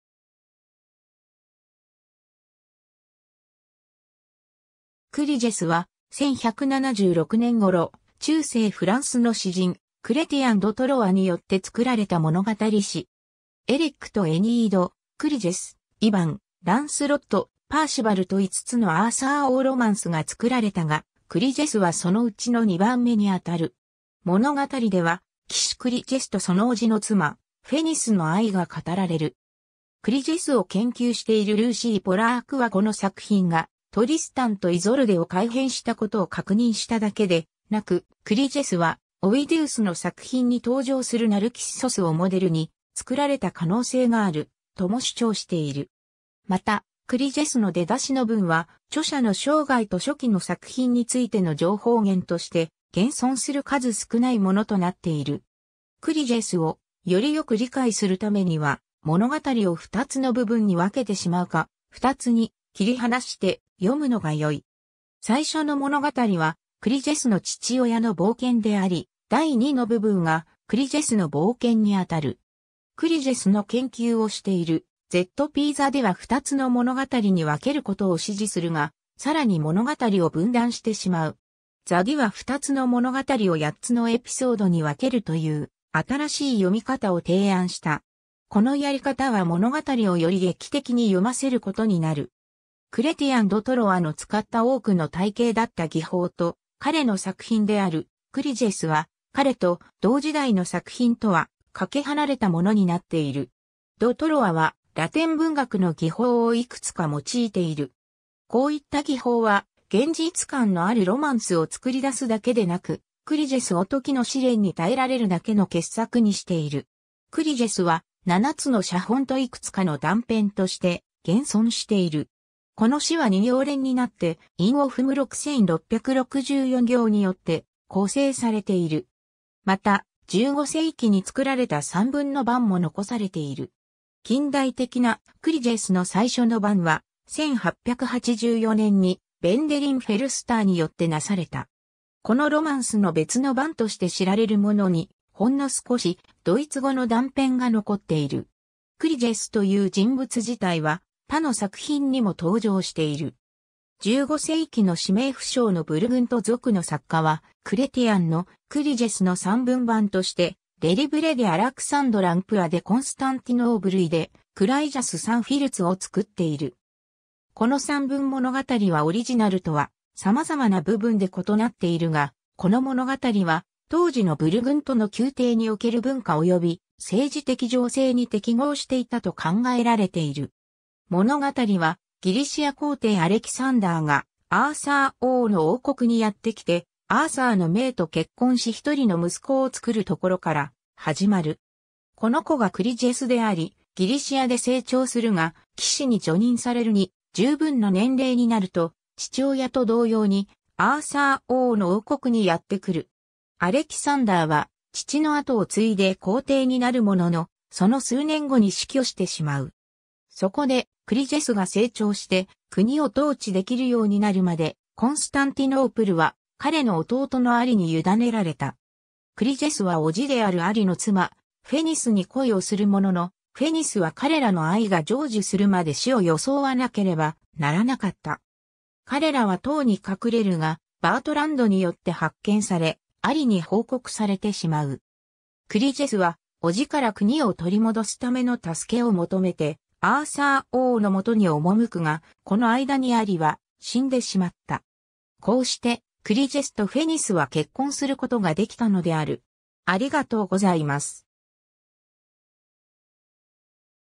クリジェス
『クリジェス』（Cligès）は、1176年ごろ、中世フランスの詩人クレティアン・ド・トロワによって作られた物語詩。
 エレックとエニード、クリジェス、イヴァン、ランスロット、パーシヴァルと5つのアーサー王ロマンスが作られたが、クリジェスはそのうちの2番目にあたる。
物語では、騎士クリジェスとその叔父の妻、フェニスの愛が語られる。
クリジェスを研究しているルーシー・ポラークはこの作品が『トリスタンとイゾルデ』を改変したことを確認しただけでなく、クリジェスはオウィディウスの作品に登場するナルキッソスをモデルに作られた可能性がある、とも主張している。
また、クリジェスの出だしの文は、著者の生涯と初期の作品についての情報源として現存する数少ないものとなっている。
 『クリジェス』をよりよく理解するためには、物語を2つの部分に分けてしまうか、2つに切り離して読むのが良い。
最初の物語はクリジェスの父親の冒険であり、第2の部分がクリジェスの冒険に当たる。
『クリジェス』の研究をしているZ.P.ザディは2つの物語に分けることを支持するが、さらに物語を分断してしまう。
ザディは2つの物語を8つのエピソードに分けると言う新しい読み方を提案した。
このやり方は物語をより劇的に読ませることになる。
 クレティアン・ド・トロワの使った多くの体系だった技法と、彼の作品である『クリジェス』は彼と同時代の作品とはかけ離れたものになっている。
ド・トロワはラテン文学の技法をいくつか用いている。
こういった技法は現実感のあるロマンスを作り出すだけでなく、『クリジェス』を時の試練に耐えられるだけの傑作にしている。
 『クリジェス』は、7つの写本といくつかの断片として現存している。
この詩は二行連になって韻を踏む6,664行によって構成されている。
また、15世紀に作られた散文の版も残されている。
近代的な『クリジェス』の最初の版は、1884年にヴェンデリン・フェルスターによってなされた。
 このロマンスの別の版として知られるものに、ほんの少しドイツ語の断片が残っている。
クリジェスという人物自体は、他の作品にも登場している。
15世紀の氏名不詳のブルグント族の作家は、クレティアンの『クリジェス』の散文版として『Le Livre de Alixandre Empereur de Constentinoble et de Cligés Son Filz』（直訳:コンスタンティノープル皇帝アレクサンダーとフィッツの子クリジェスの生涯）を作っている。
この散文物語はオリジナルとはさまざまな部分で異なっているが、この物語は当時のブルグントの宮廷における文化および政治的情勢に適合していたと考えられている。
 物語は、ギリシア皇帝アレキサンダーが、アーサー王の王国にやってきて、アーサーの姪と結婚し1人の息子を作るところから始まる。
 この子がクリジェスであり、ギリシアで成長するが、騎士に叙任されるに充分な年齢になると、父親と同様にアーサー王の王国にやってくる。
アレキサンダーは父の後をついで皇帝になるものの、その数年後に死去してしまう。
そこで、クリジェスが成長して国を統治できるようになるまで、コンスタンティノープルは彼の弟のアリに委ねられた。
クリジェスは叔父であるアリの妻、フェニスに恋をするものの、フェニスは彼らの愛が成就するまで死を装わなければならなかった。
彼らは塔に隠れるが、バートランドによって発見され、アリに報告されてしまう。
クリジェスは叔父から国を取り戻すための助けを求めてアーサー王の元に赴くが、この間にアリは死んでしまった。
こうしてクリジェスとフェニスは結婚することができたのである。